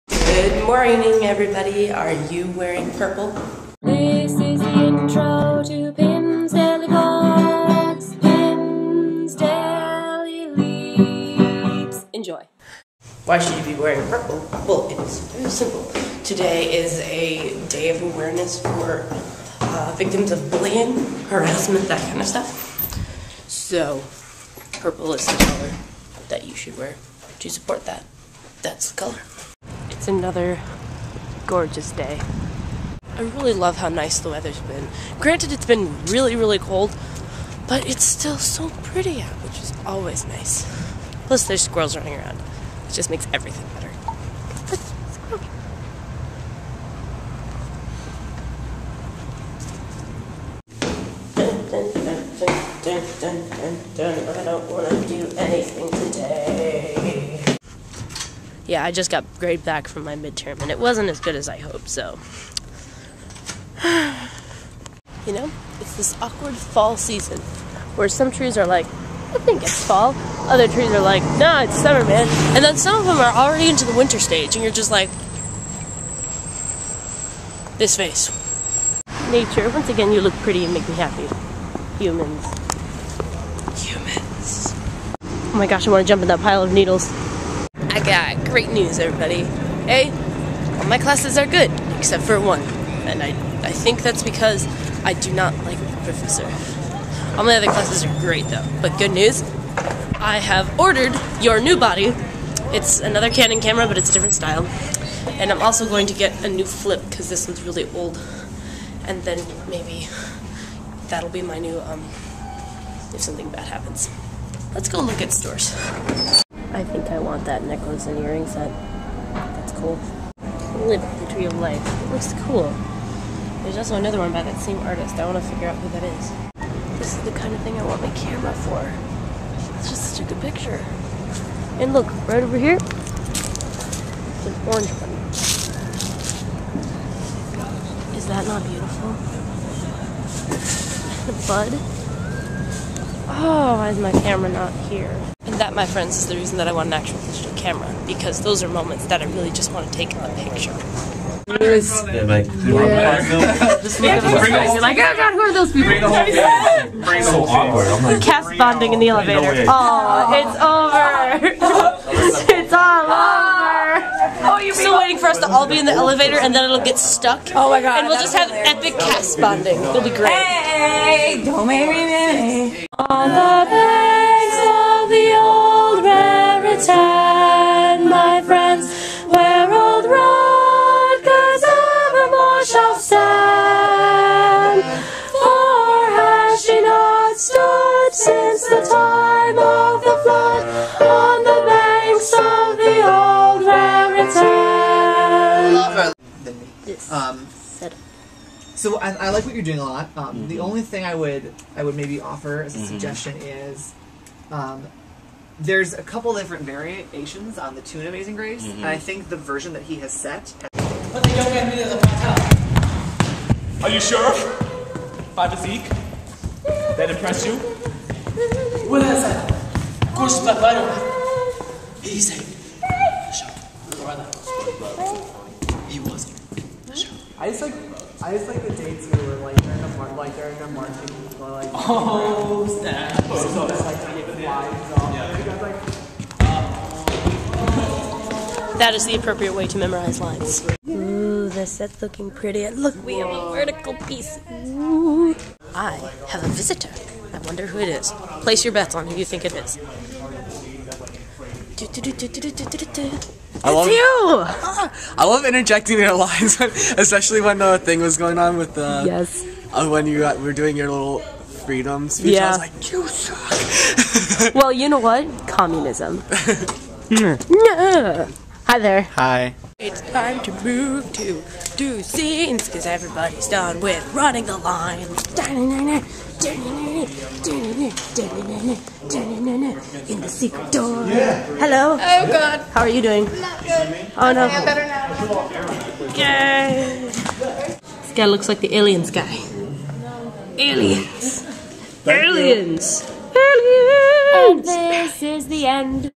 Cast Bonding {Day 291}
Did you know that today is a day of awareness for people who have been bullied?  It's a day to wear purple to support those who have been lost and those who are still being hurt.  Are you wearing purple?

Meanwhile, the Robeson cast bonds even more within the tight space of an elevator, the fall season brings reason for still pictures and squirrels inherit the earth.